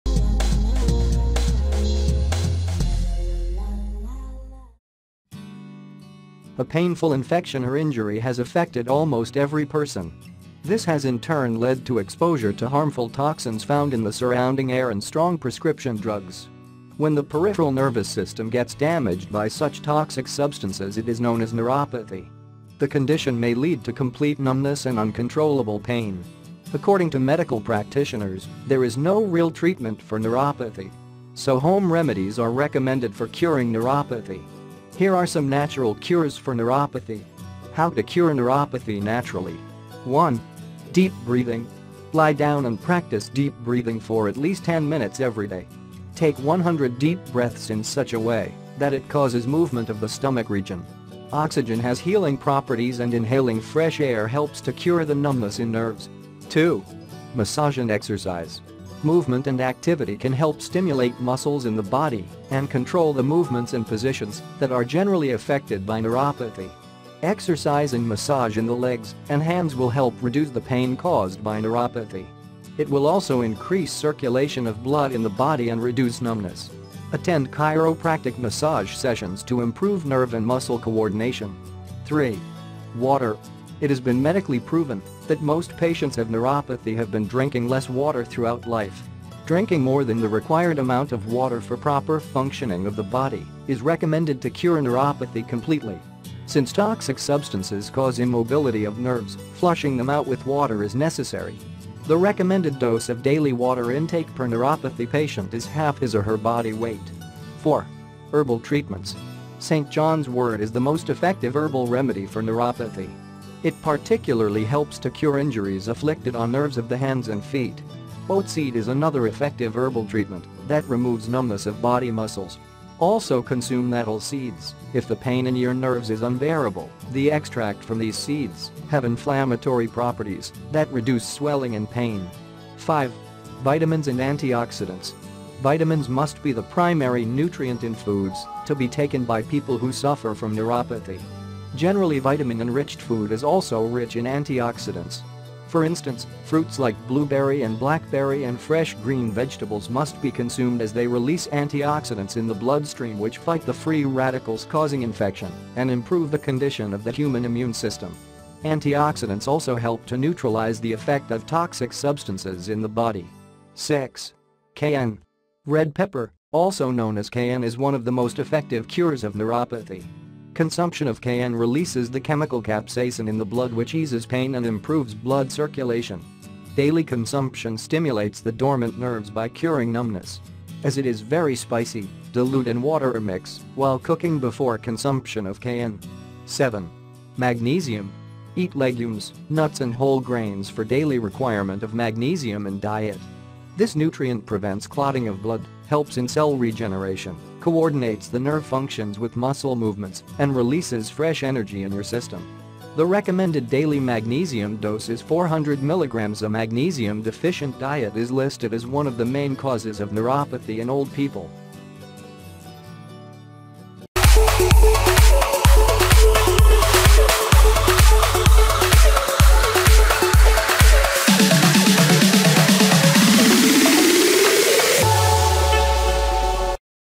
How To Cure Neuropathy Naturally | Natural Remedies For Neuropathy
7 Natural Remedies For Neuropathy

A painful infection or injury has affected almost every person. This has in turn led to exposure to harmful toxins found in the surrounding air and strong prescription drugs.

When the peripheral nervous system gets damaged by such toxic substances it is known as neuropathy. The condition may lead to complete numbness and uncontrollable pain.

According to medical practitioners, there is no real treatment for neuropathy. So home remedies are recommended for curing neuropathy. Here are some natural cures for neuropathy.
How To Cure Neuropathy Naturally.